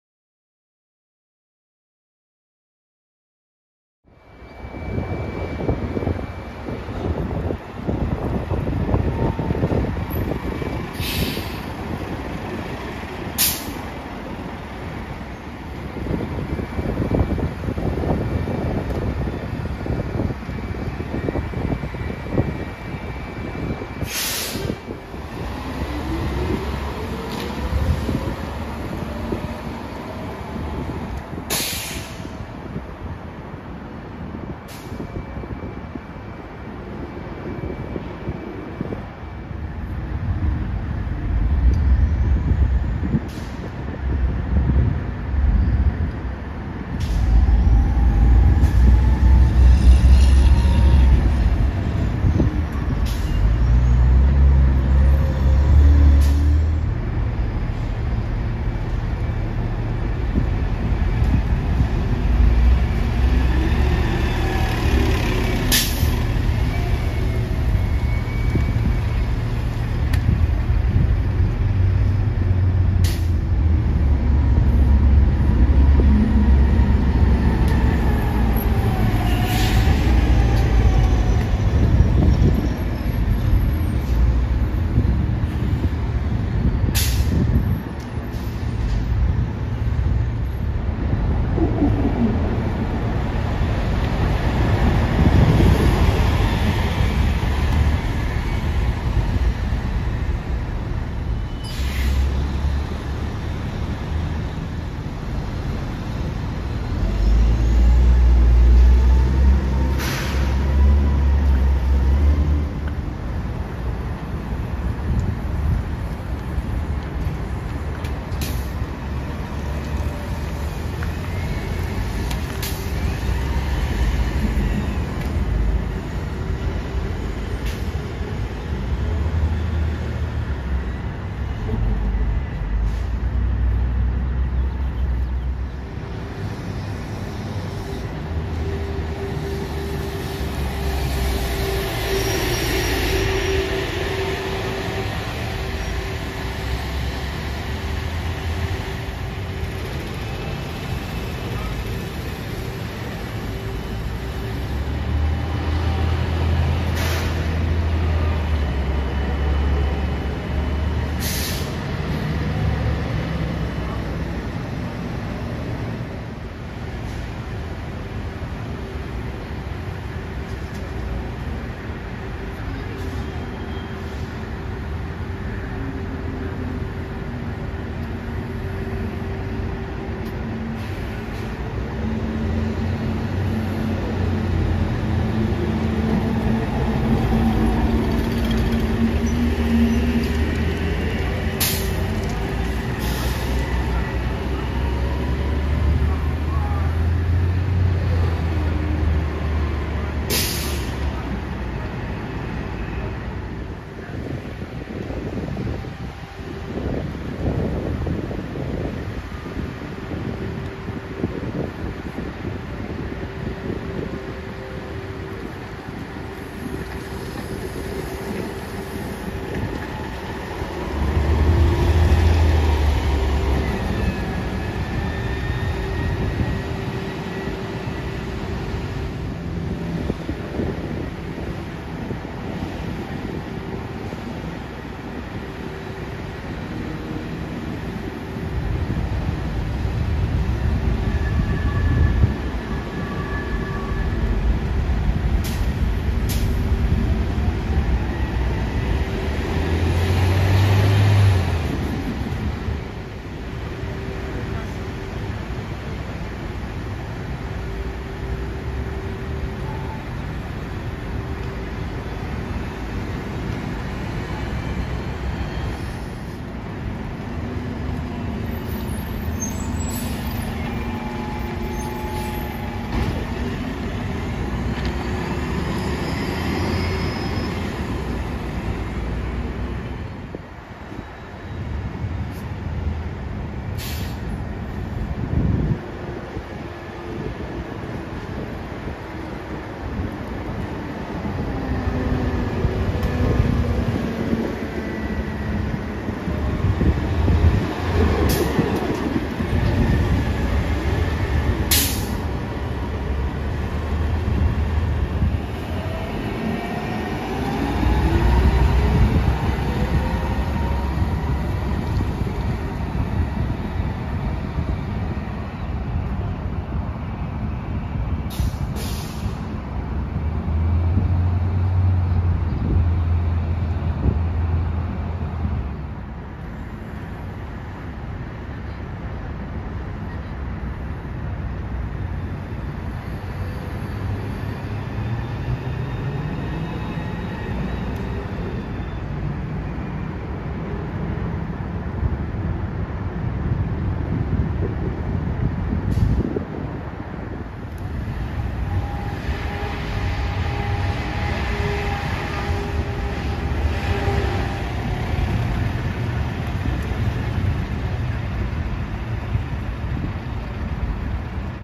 ⁴ᴷ⁶⁰ Transit Action EP08 | LA Metro, Culver, LAX, Big Blue Buses @ Aviation/LAX Station
Video Category: Transit Action
Episode: 08
Location: Aviation/LAX Station

Routes:
LA Metro 857
Culver Citybus 6
LAX Shuttle M
Big Blue Bus 3

Bus Models:
New Flyer C40LFR
New Flyer Xcelsior XN40
New Flyer Xcelsior XN60
Gillig BRT CNG 40'
VMC Vicinity Classic 35' CNG

Second SoCal video!

Welcome to Aviation/LAX Station, a popular transit hub that connects to/from Los Angeles International Airport and the Metro Rail K Line with a bus shuttle. Big Blue Bus and Culver Citybus also provide service to Santa Monica and UCLA. This is indeed a busy station, since it connects directly to the LA Metro Rail C line, which serves a lot of popular destination and the rest of the LA Metro Rail system.